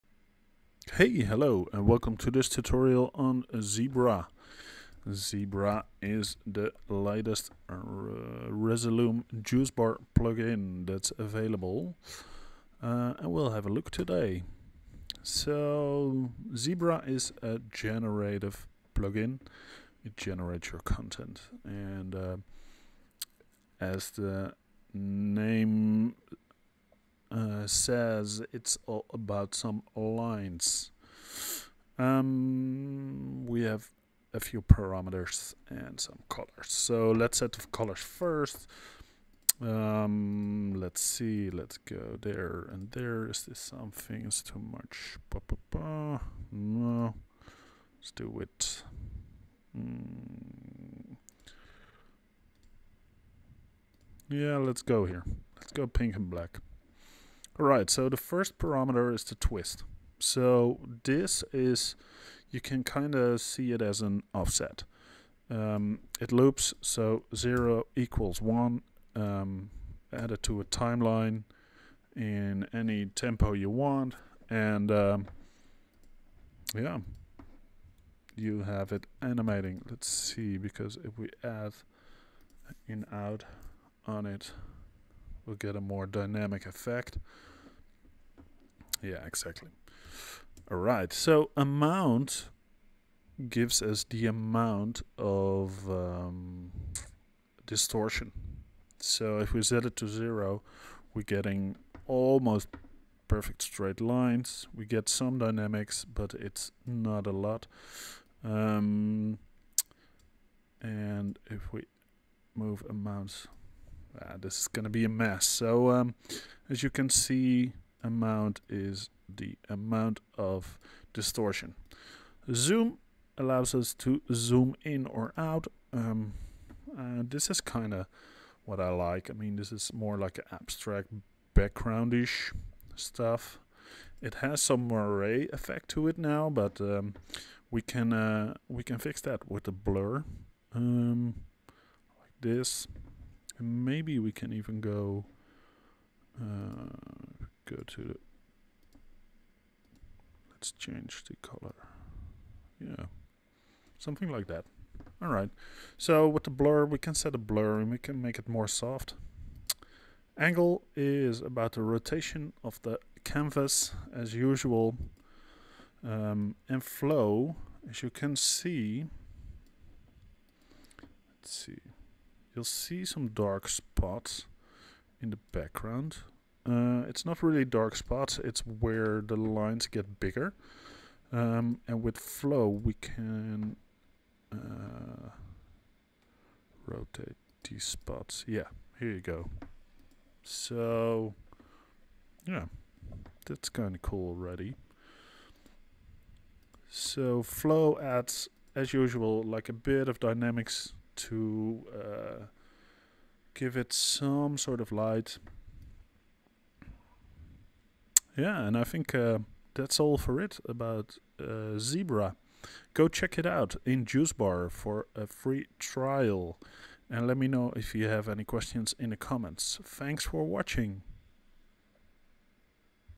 Resolume Plugin: Zebra
This video show you how to use the Zebra plugin in Resolume with JuiceBar.

Zebra gives you the famous two stripes, in multiple flavours.